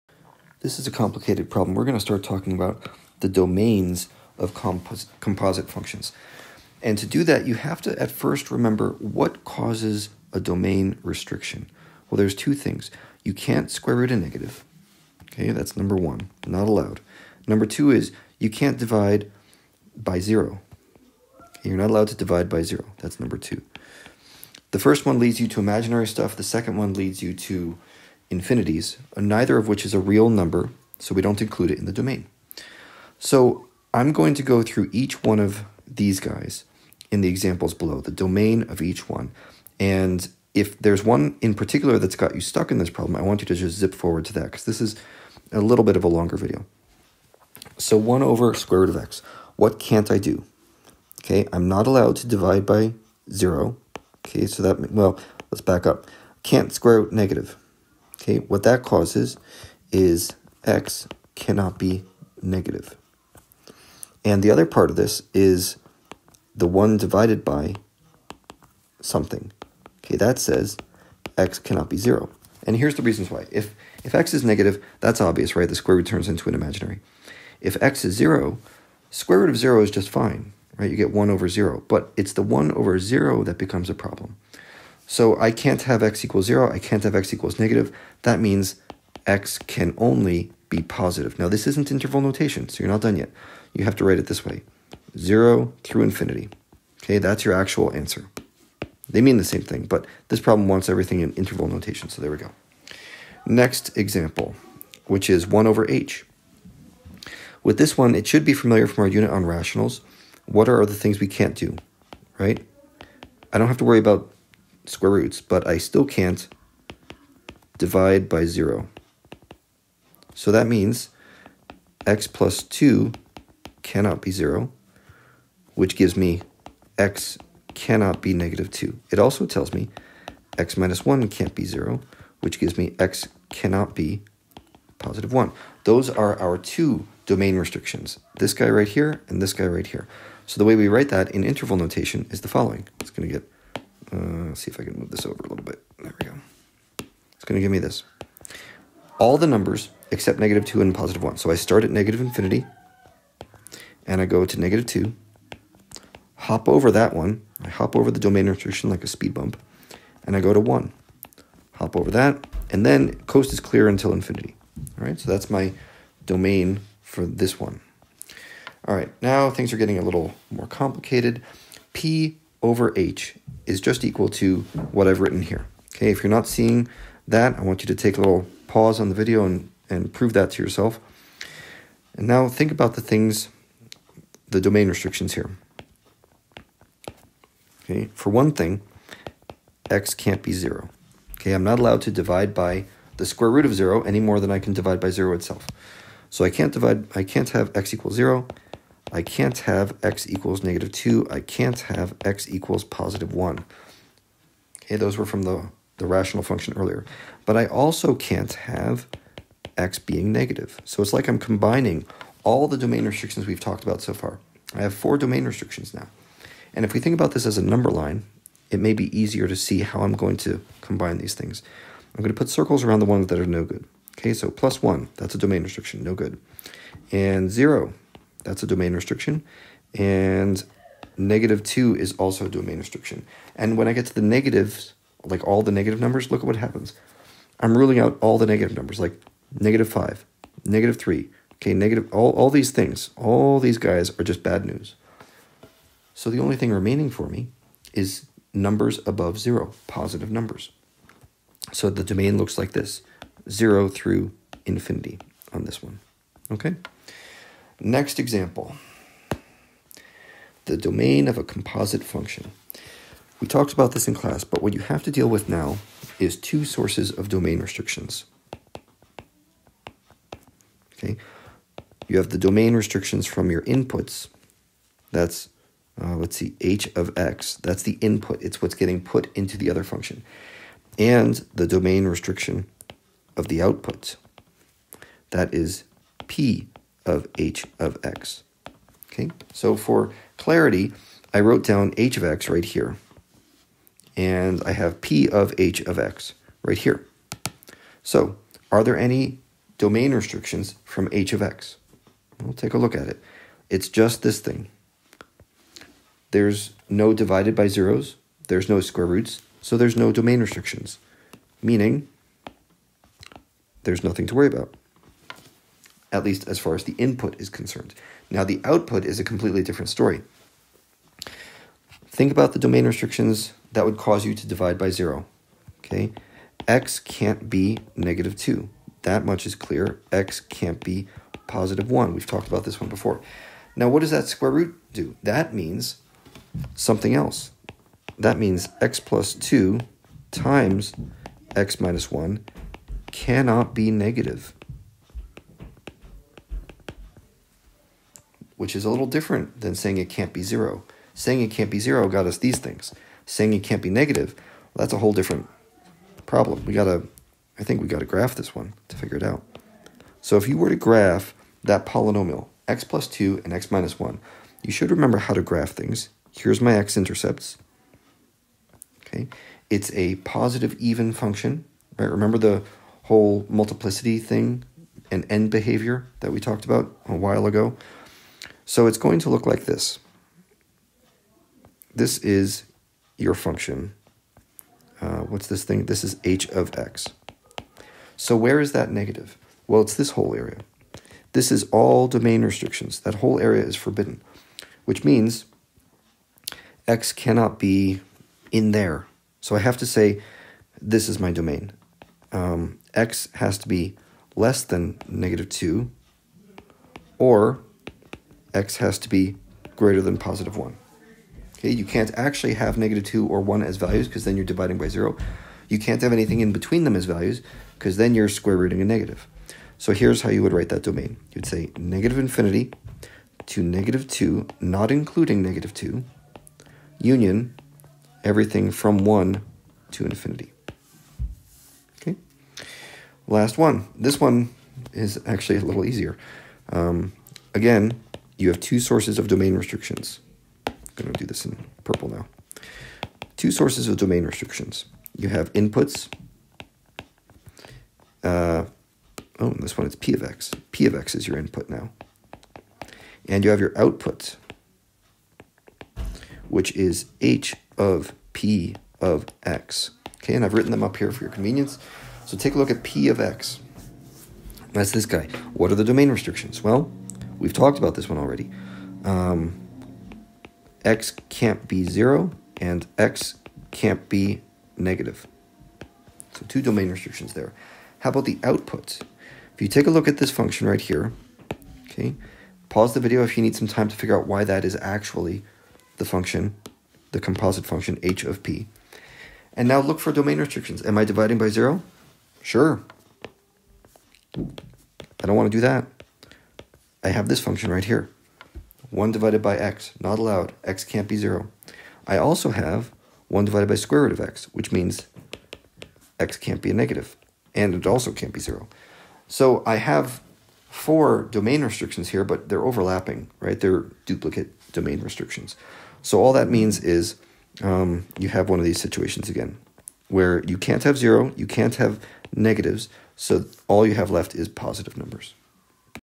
SM3.05.25 - Domain of Composite Functions
Murphy Math --
Introduction to finding the domain of composite functions.  Example shown is composition of a reciprocal square root function with a polynomial function.  Covers the domain restrictions produced by the composition and by the input function.  Includes review of writing domain interval notation using domain restrictions.  Domain restriction of composite function is found using graphing techniques.